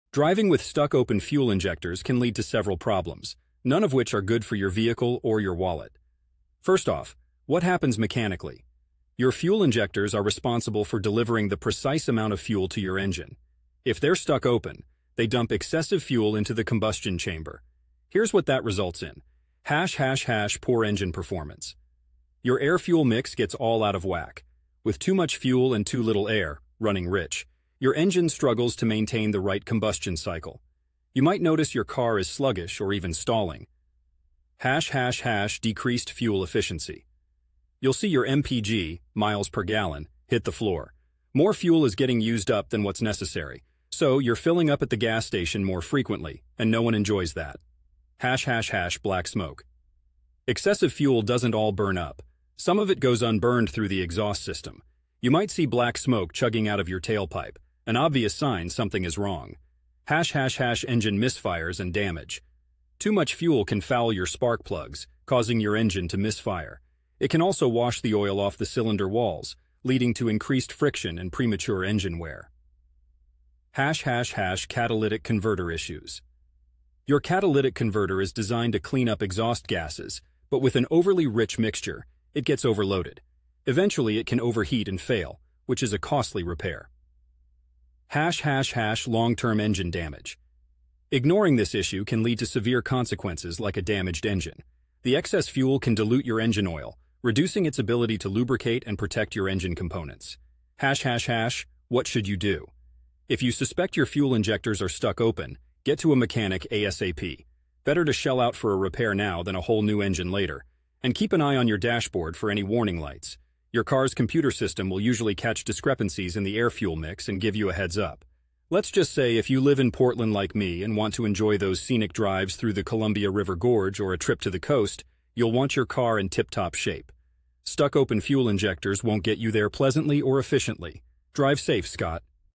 What Driving with Open Fuel Injectors Does to Your Car!
I answered this question on Quora My video answer to the question: What Happens If You Drive With Stuck Open Fuel Injectors?